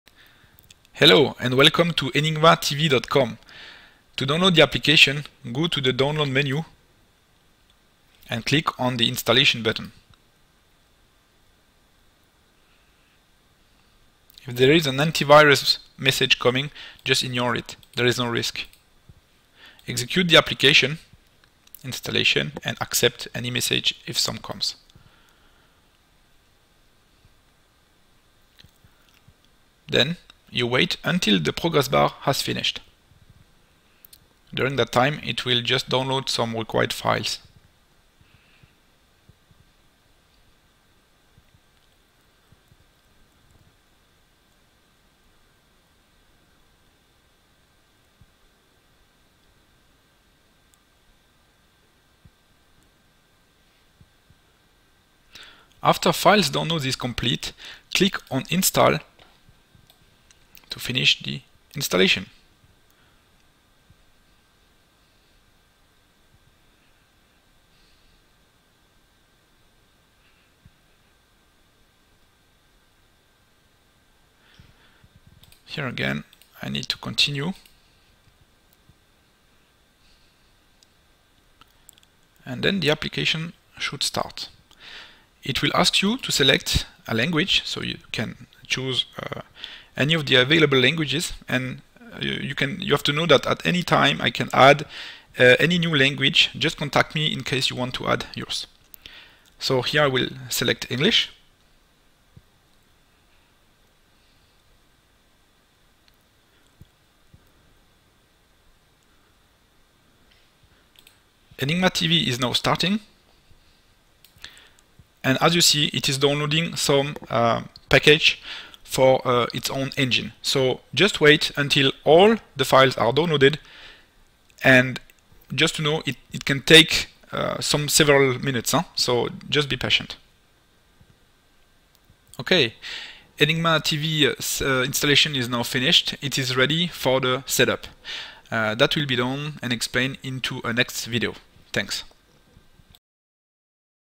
Enigma-TV Step 1 : Installation
Explanation on Enigma-TV installation process.